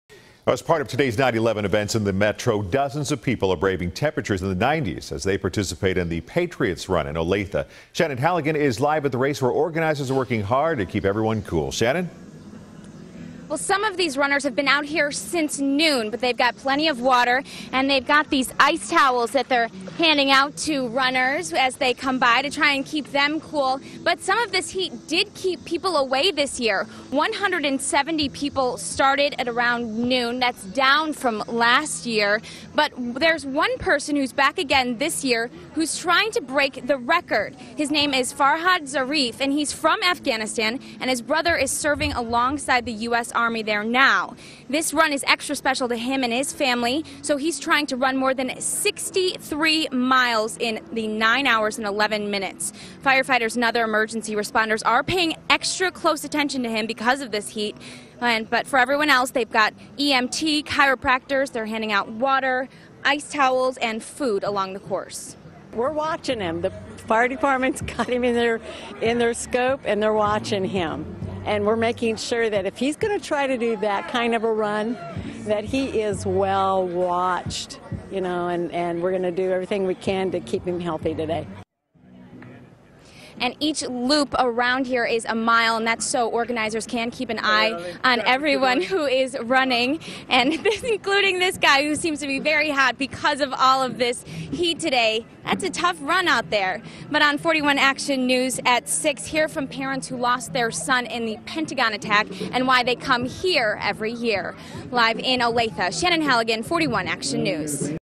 Heat doesn't stop runners in Olathe's Patriot Run
Dozens of people braved temperatures in the 90s as they participated in the Patriot Run.